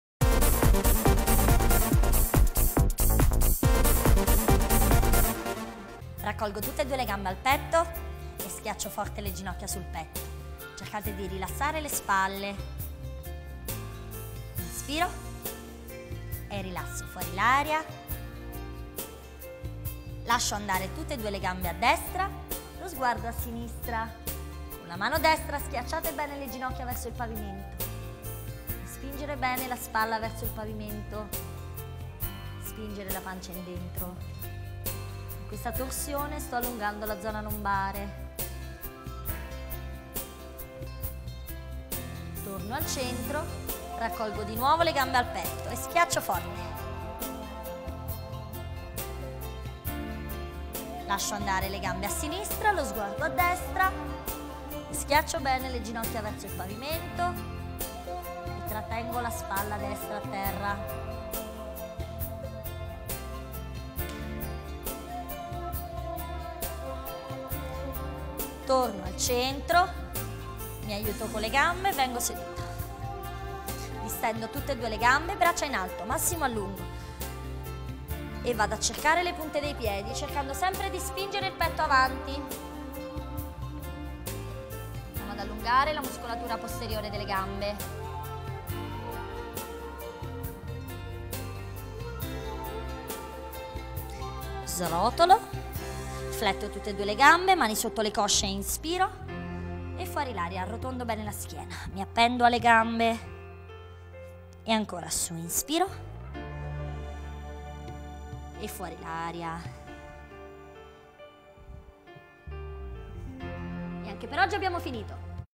Fitness: esercizi di stretching
Una nuova lezione di fitness con Giovanna Lecis, che ci mostra come eseguire gli esercizi di stretching. Altre video lezioni di fitness su